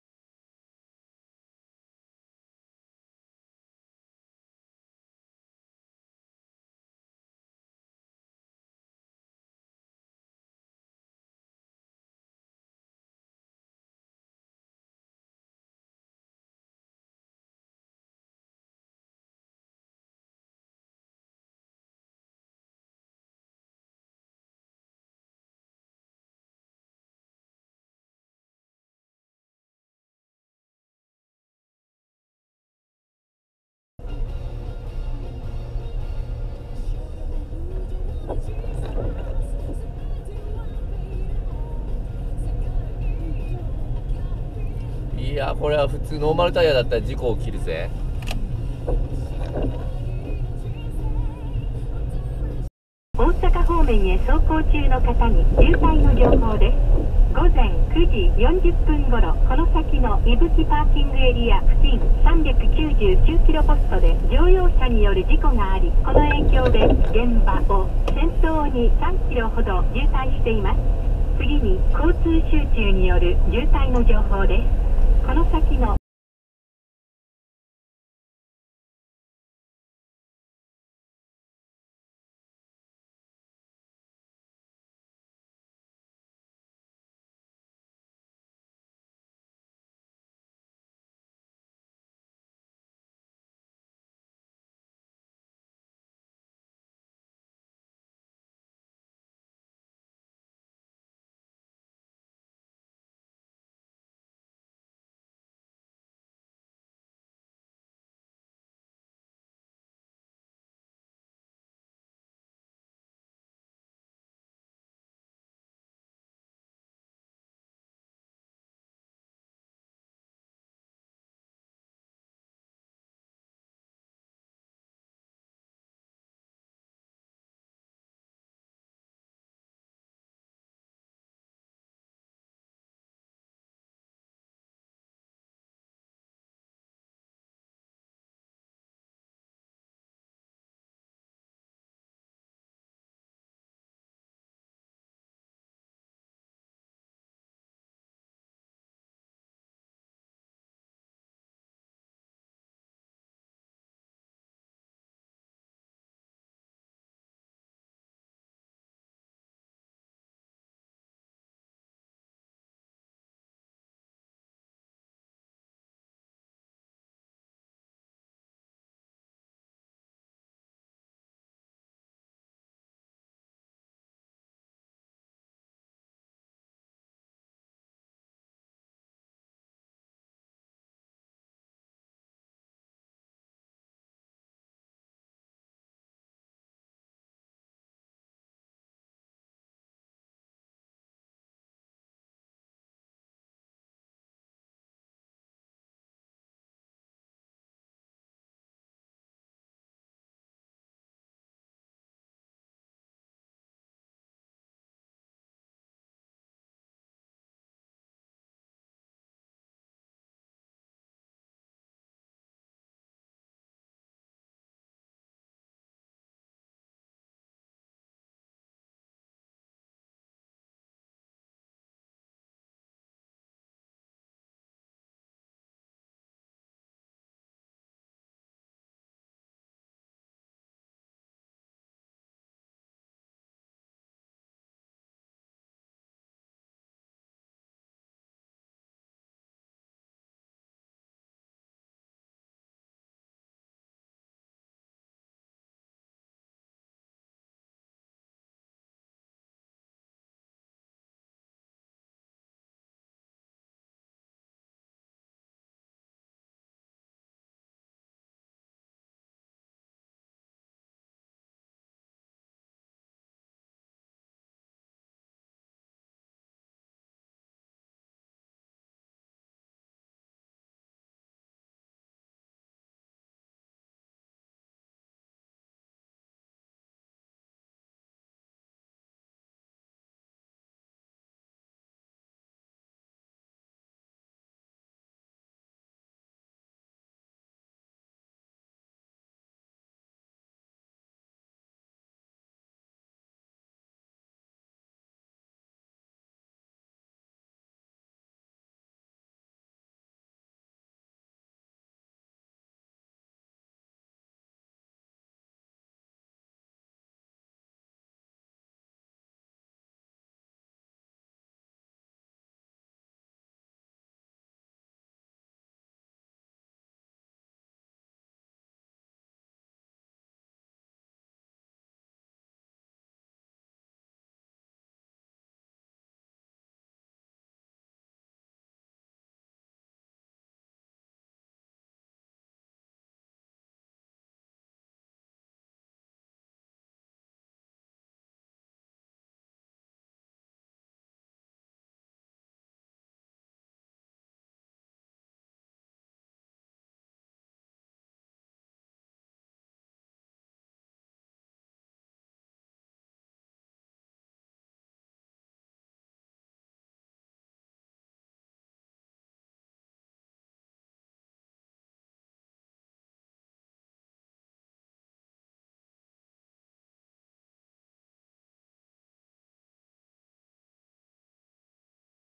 大雪の名神高速 関ヶ原IC 伊吹PA 帰省ラッシュを直撃 デリカD:5 スタッドレスタイヤで雪の高速道路走行
2013年12月28日。帰省ラッシュで混雑する名神高速道路に雪が降る。名神高速道路下り線、関ヶ原IC付近から雪が多くなり、関ヶ原、伊吹PA付近は路面に積雪。スリップによる事故が多発して渋滞が発生。チェーン規制や通行規制はされていないので、ノーマルタイヤで走る車による事故が多く発生した。
デリカD:5、4WD固定、スタッドレスタイヤ（冬用タイヤ）
GoProHD Hero3